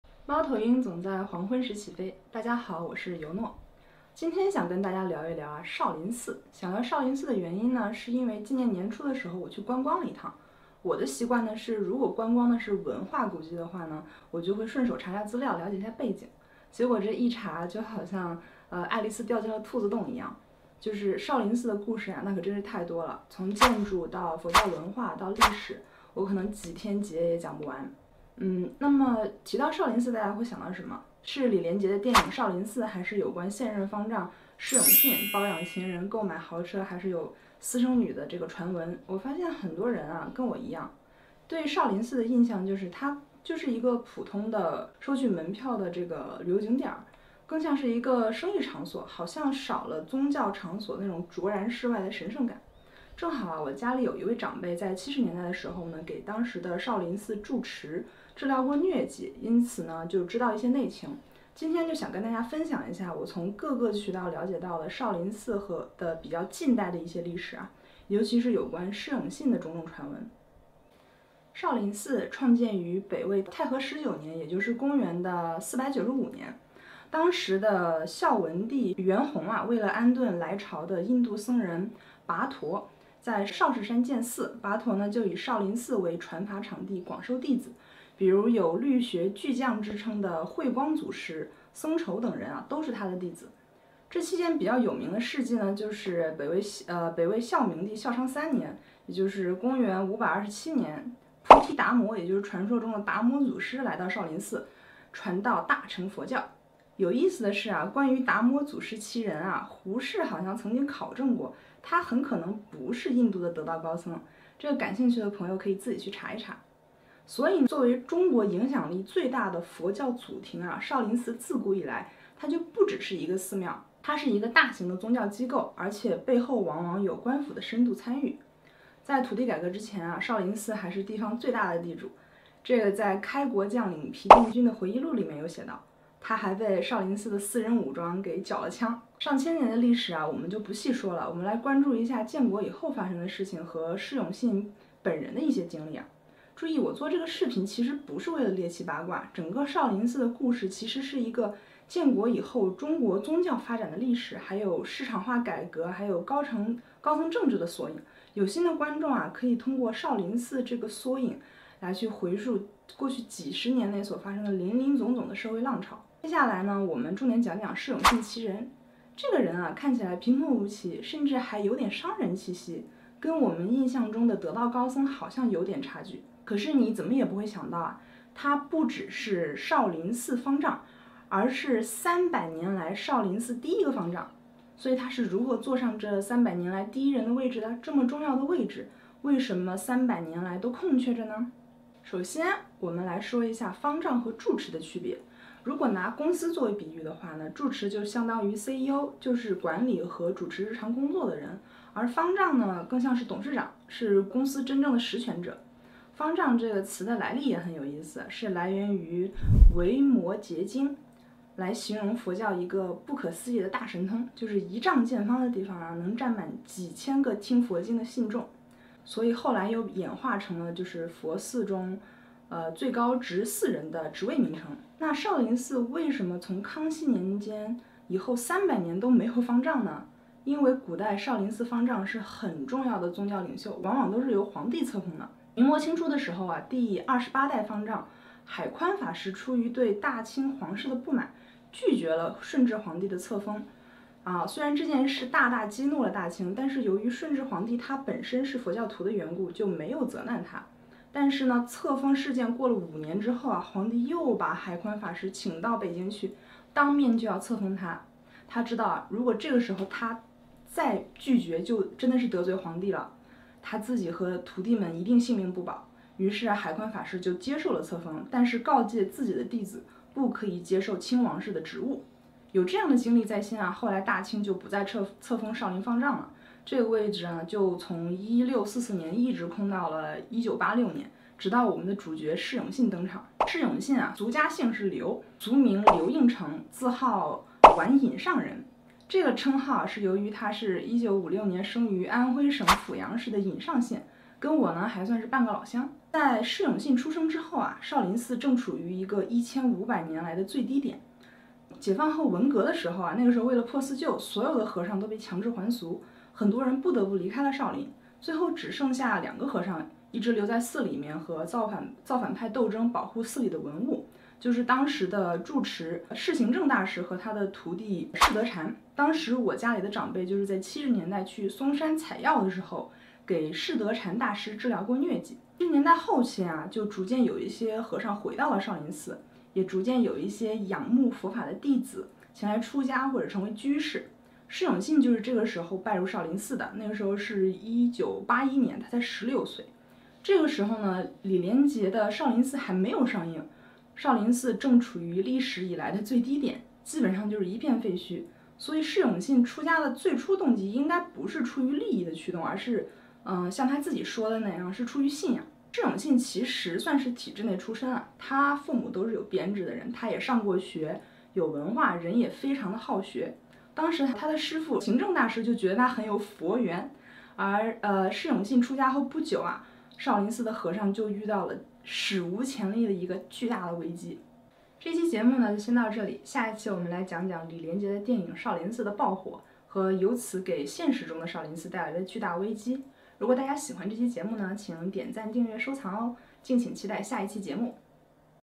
少林寺的真实故事，比电影里精彩多了（上集）
大家好，尤诺又带着历史干货来了！这次要聊的是大家耳熟能详的少林寺。

很多人看过和少林寺有关的武打电影，但尤诺发现，真实的少林寺山门里的故事可比那些电影精彩多了。这些故事并不简单是八卦消息，少林寺的沉浮可以说体现了中国近代社会的时代变迁。

本期是少林寺系列的第一集，主要介绍的是少林寺建国前的历史，以及现任方丈释永信的出身。后面的内容在剪辑中，敬请期待！

文案：zhimeng/尤诺
剪辑：tianyue
摄影：tianyue
制作：Cahey